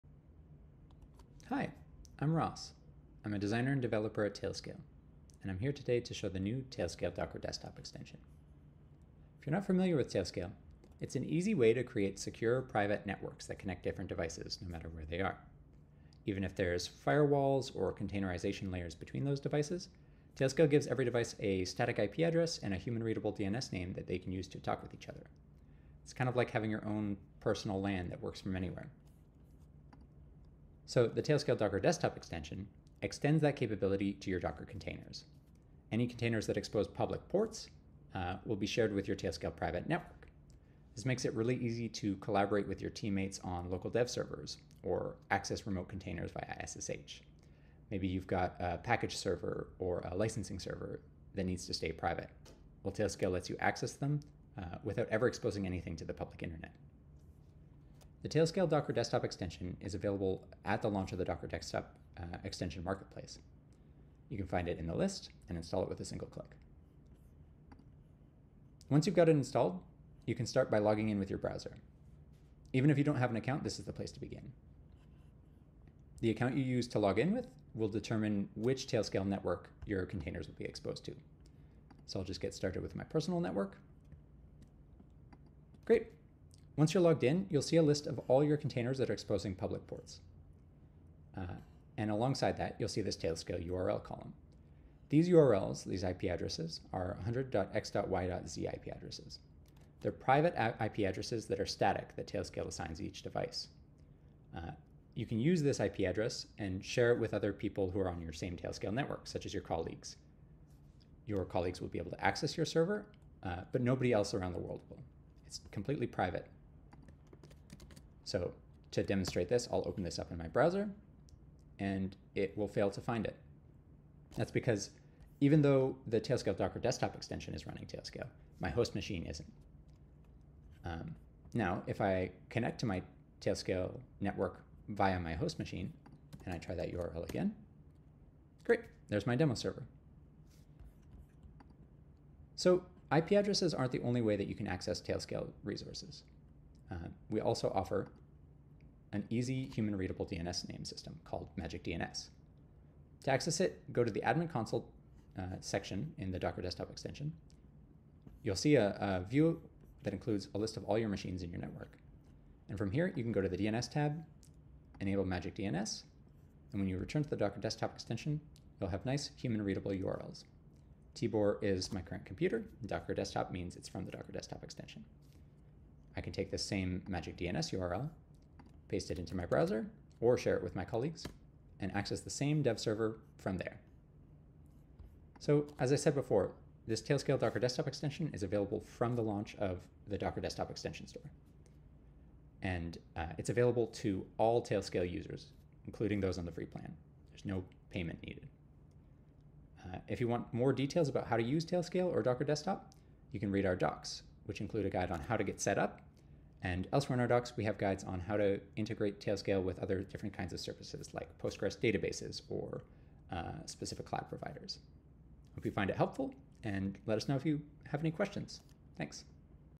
Tailscale Docker Desktop Extension
With the Tailscale Docker Desktop extension, you can use Tailscale to securely connect to the resources you need for development, including internal tools and databases, no matter where you are or where your development environment lives.